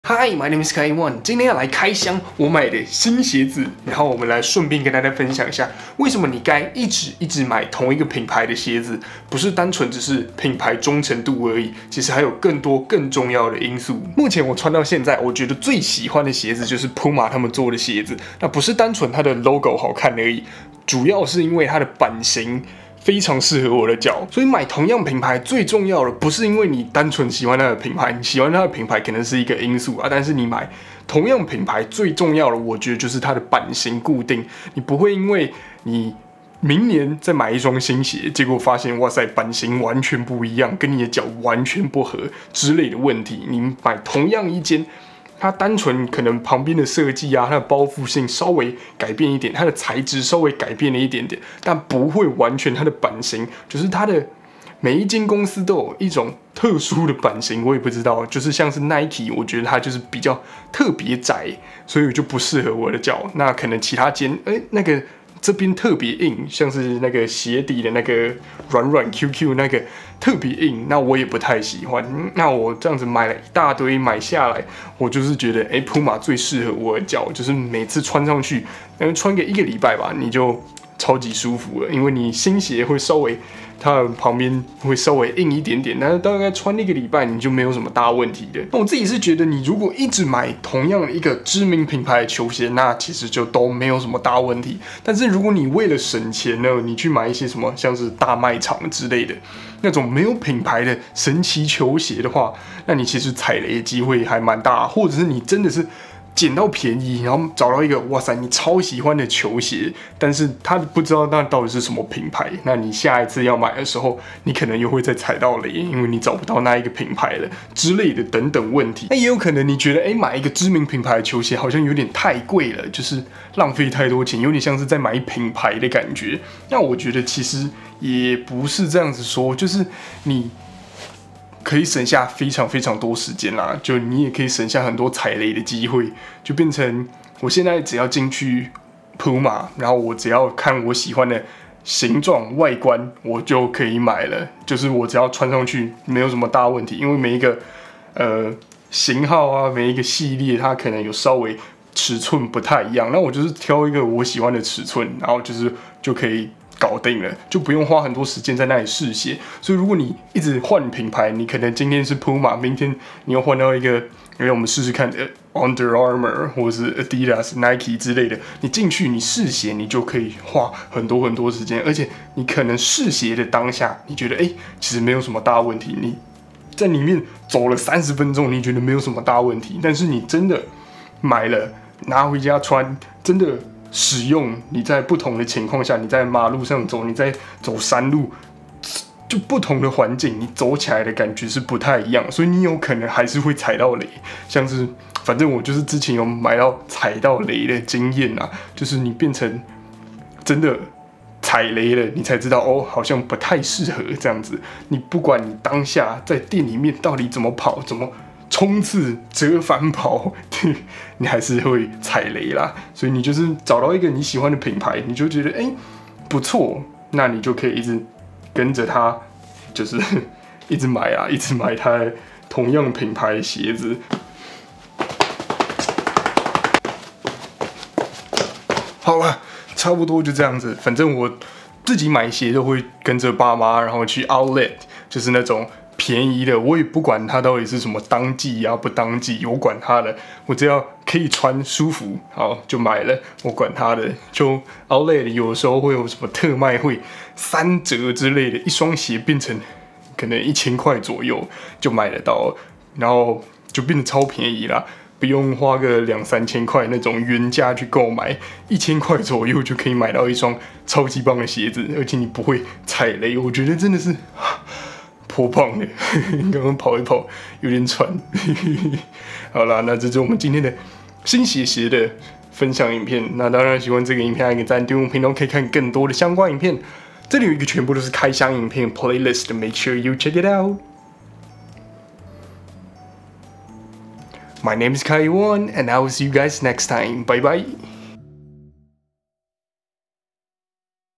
Outlet 採購3折鞋子 PUMA 運動鞋開箱 為什麼要買知名品牌的球鞋？固定品牌忠實顧客
適合的鞋子最重要, 但你鞋子一定會換, 固定品牌才有可能辦到穩定的版型, 讓你省下不少時間
歡迎加入社群